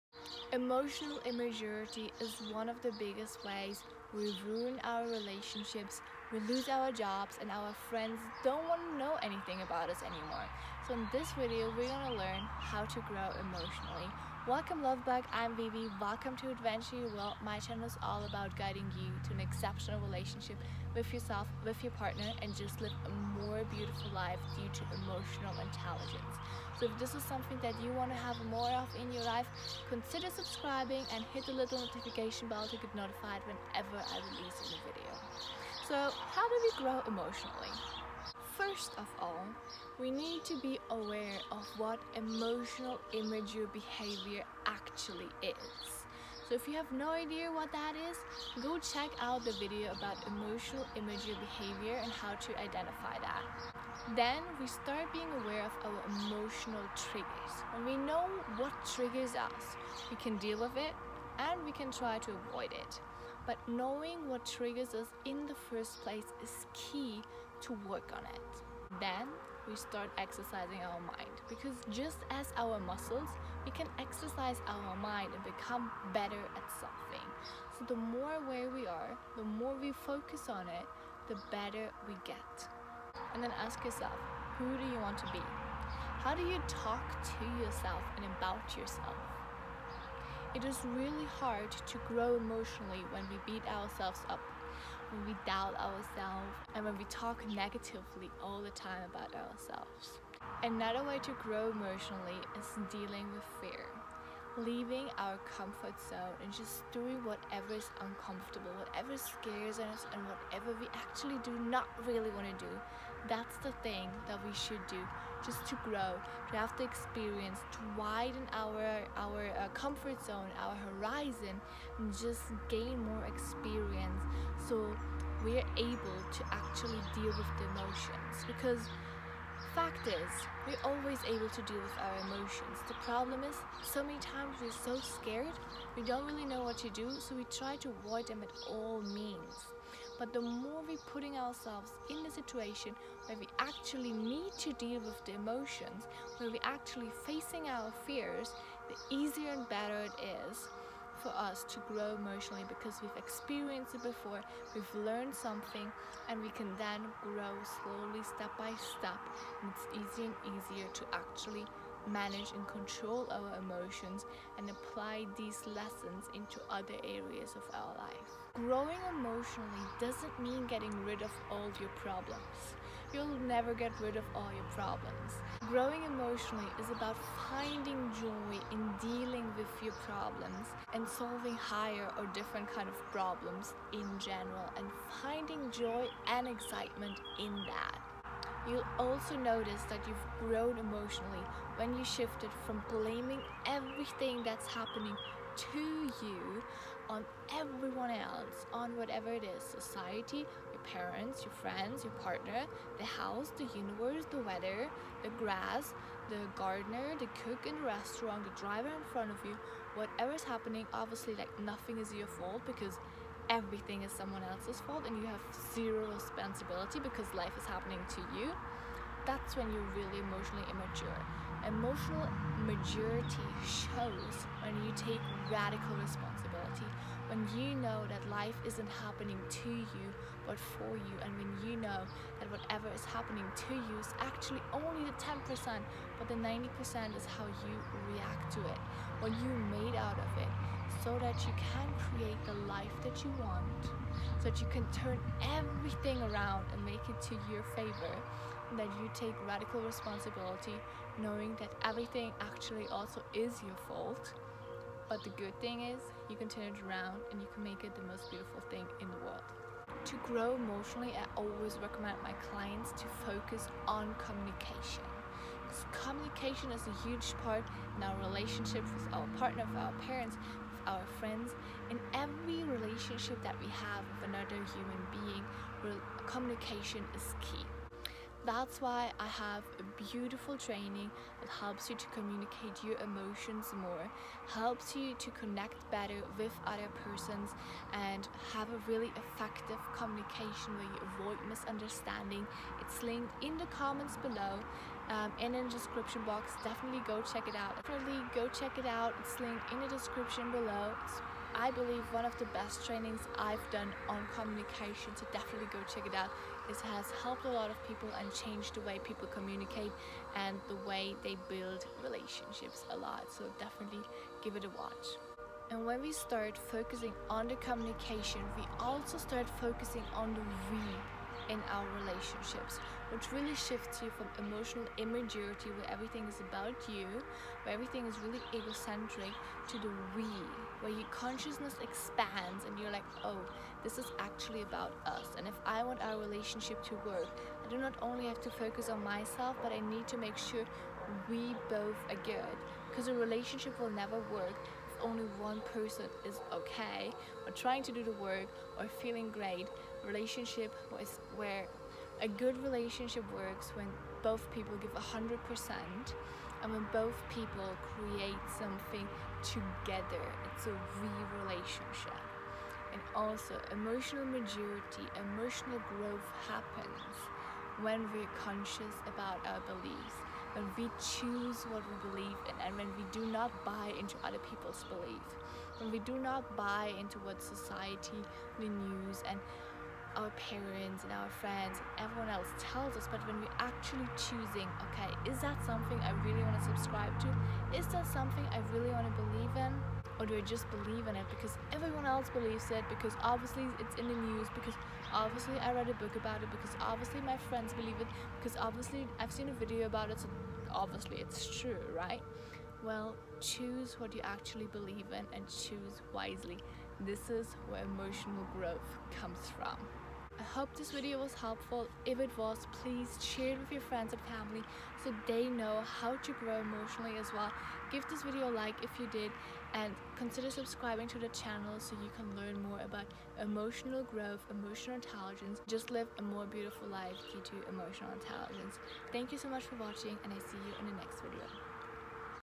Growing Emotionally - how to grow emotionally with ease
Get the free Communication Training to support your emotional growth and create better & deeper relationships here:

In This video Vivi from Adventure Your World talks about Growing Emotionally - how to grow emotionally with ease. Emotional Growth is a vital part in self-mastery as our emotions create our reality and either push us to the life we dream of living or hold us back and let us play small. Emotional growth helps us improve the quality of our life.

If you are interested in 1:1 Coaching or finding out which of my courses fits you best either fill out this form here

This Video is about Growing Emotionally - how to grow emotionally with ease